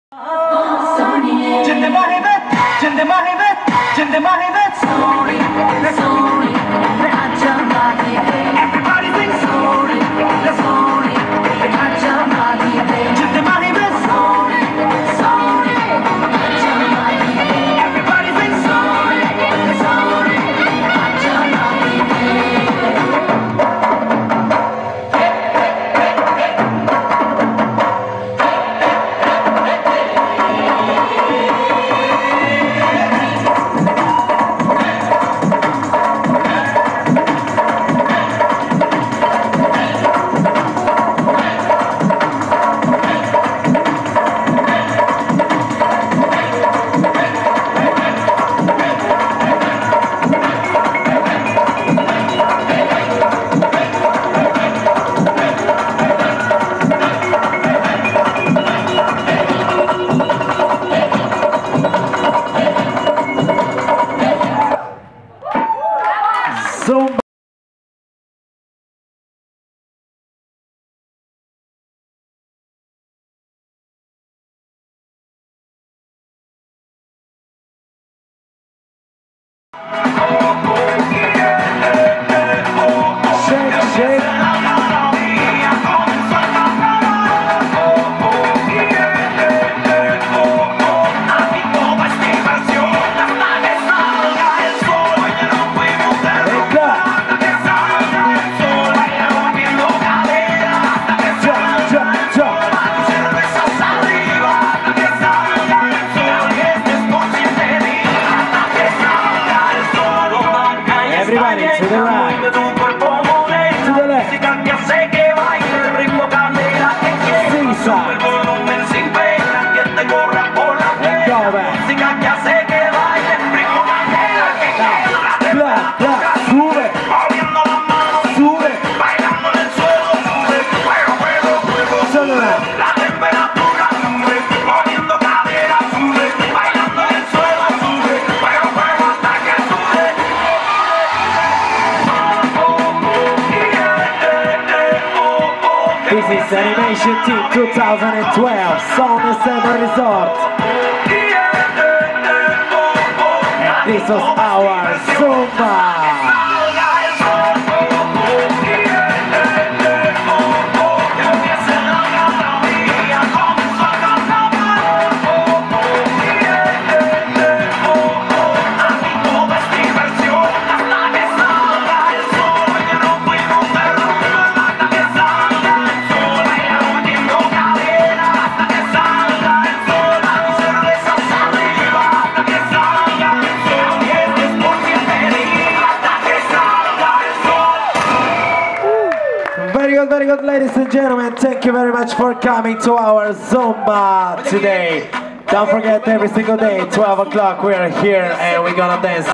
Hotel Sol Nessebar Mare 4* - cursuri de dans ora 12.00 ZUMBA
Hotel Sol Nessebar Mare 4* - cursuri de dans ora 12.00
Statiunea Nessebar, Bulgaria.